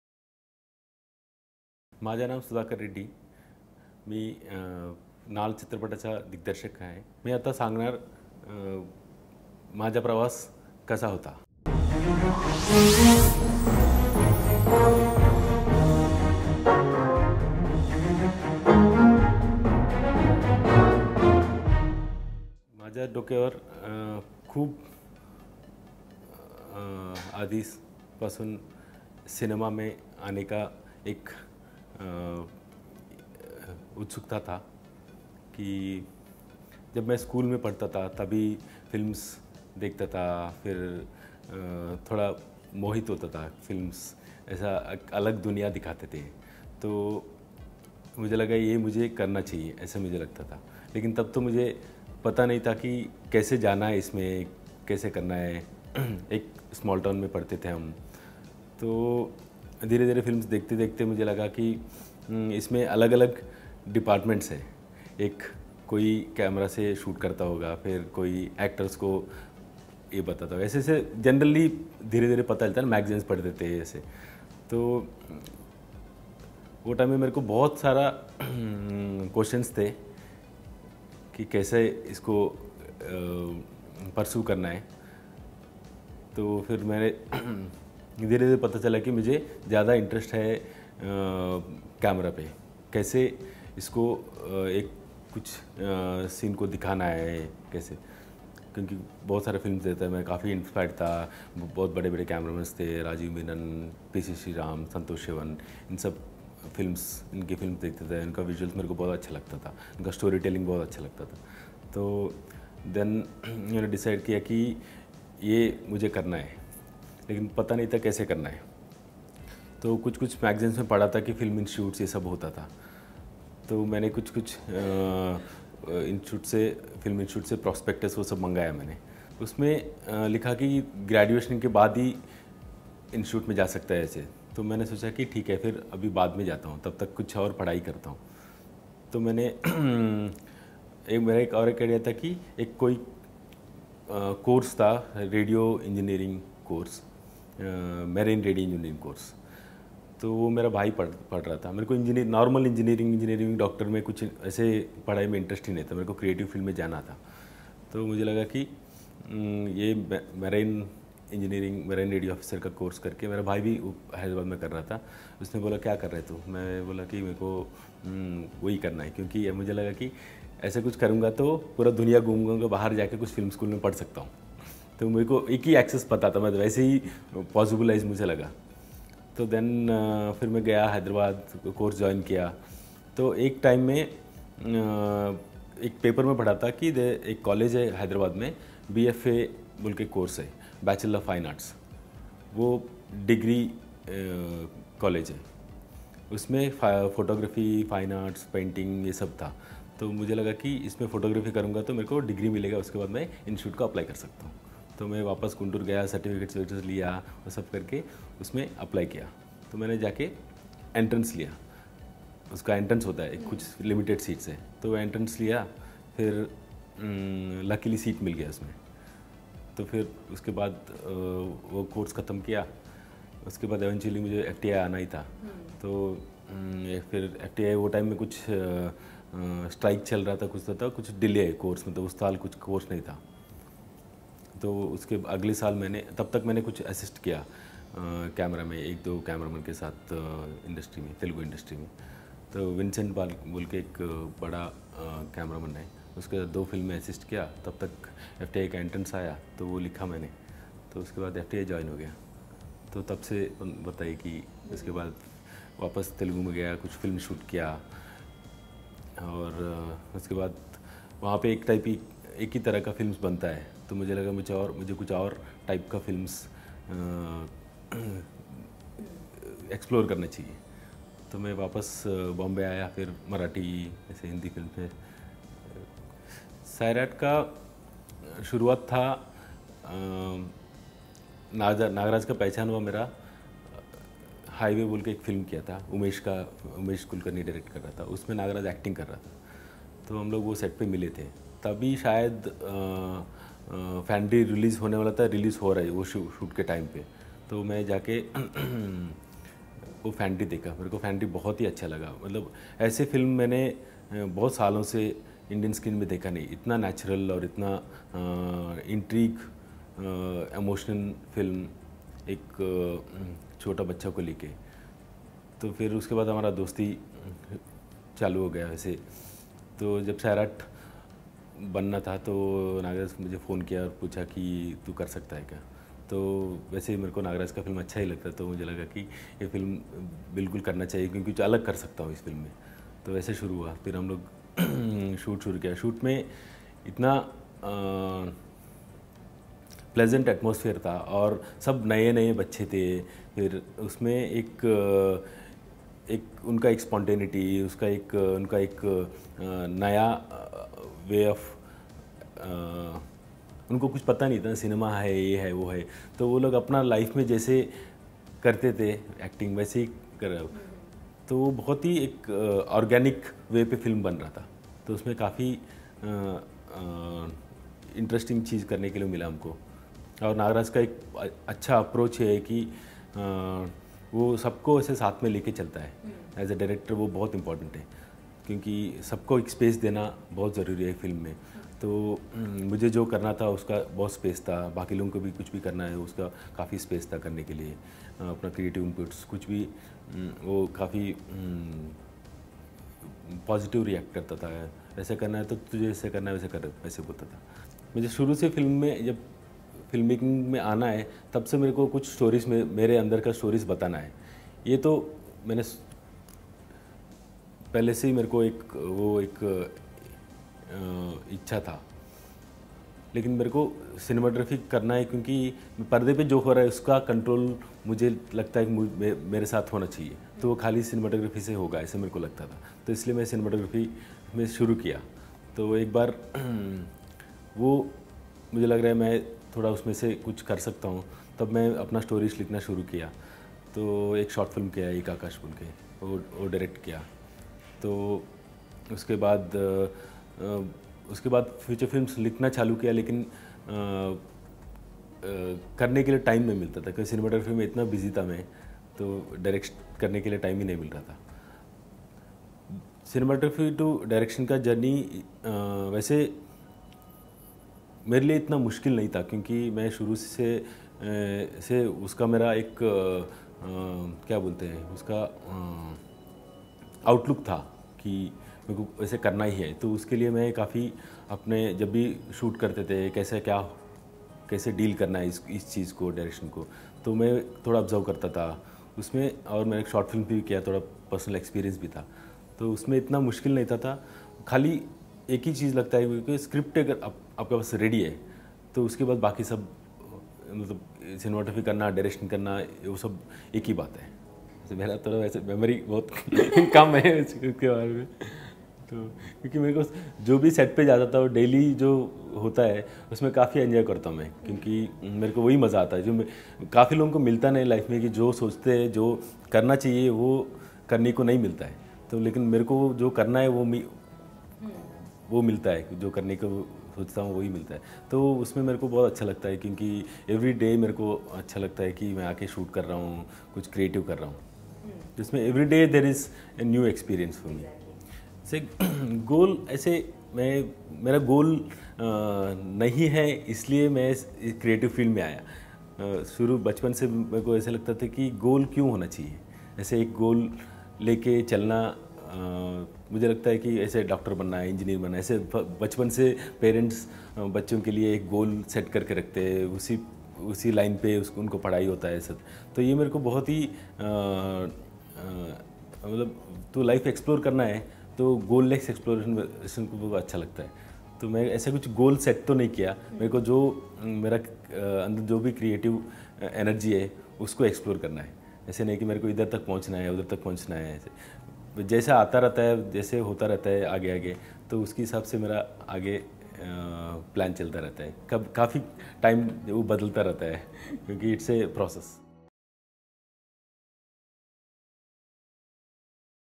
Naal | Director's Cut | Sudhakar Reddy Yakkanti | Sairat
सैराट सिनेमाची सिनेमॅटोग्राफी ते नाळ सिनेमाचं दिग्दर्शन.. कसा होता संपूर्ण प्रवास आणि अनुभव सांगत आहेत नाळ सिनेमाचे दिग्दर्शक सुधाकर रेड्डी.  Watch his Directorial Journey In this video.